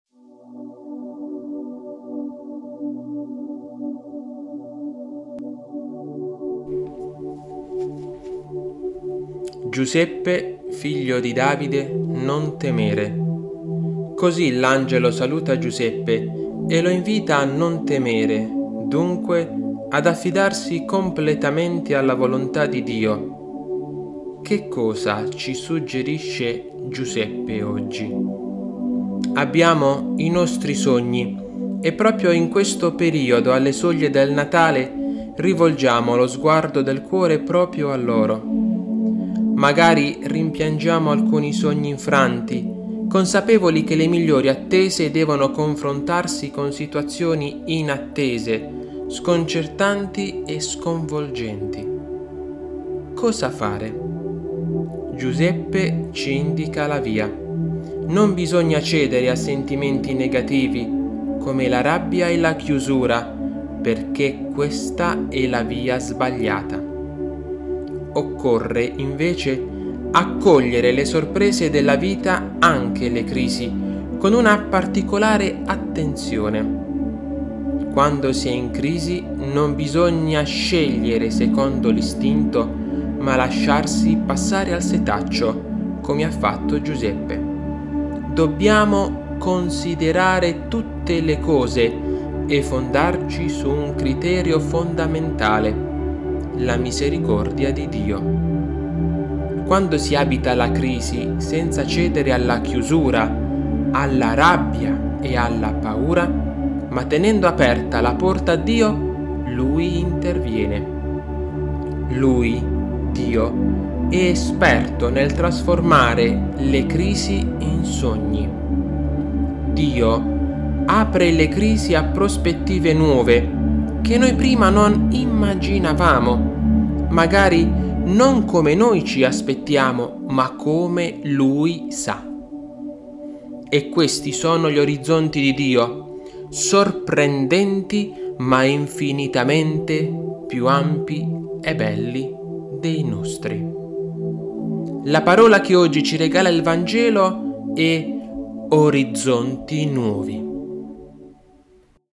📽 PAROLE DI AVVENTO 2023 [lunedì 18 dicembre]
Lunedì 18 dicembre - III settimana di Avvento

📖 Mt 1,18-24

«Giuseppe, figlio di Davide, non temere». Così l’Angelo saluta Giuseppe e lo invita a non temere, dunque ad affidarsi completamente alla volontà di Dio. Che cosa ci suggerisce Giuseppe?
Abbiamo i nostri sogni e, proprio in questo periodo, alle soglie del Natale, rivolgiamo lo sguardo del cuore proprio a loro. Magari rimpiangiamo alcuni sogni infranti, consapevoli che le migliori attese devono confrontarsi con situazioni inattese, sconcertanti, sconvolgenti.
Cosa fare? Giuseppe ci indica la via: non bisogna cedere a sentimenti negativi, come la rabbia e la chiusura, perché questa è la via sbagliata.
Occorre, invece, accogliere le sorprese della vita, anche le crisi, con una particolare attenzione: quando si è in crisi non bisogna scegliere secondo l’istinto, ma lasciarsi passare al setaccio, come ha fatto Giuseppe.
Dobbiamo “considerare tutte le cose” e fondarci su un criterio fondamentale: la misericordia di Dio.
Quando si abita la crisi senza cedere alla chiusura, alla rabbia e alla paura, ma tenendo aperta la porta a Dio, Lui interviene.
Lui è esperto nel trasformare le crisi in sogni: Dio apre le crisi a prospettive nuove, che noi prima non immaginavamo, magari non come noi ci aspettiamo, ma come Lui sa. E questi sono gli orizzonti di Dio: sorprendenti, ma infinitamente più ampi e belli dei nostri.

💡 La parola che oggi ci regala il Vangelo è
➡  ORIZZONTI NUOVI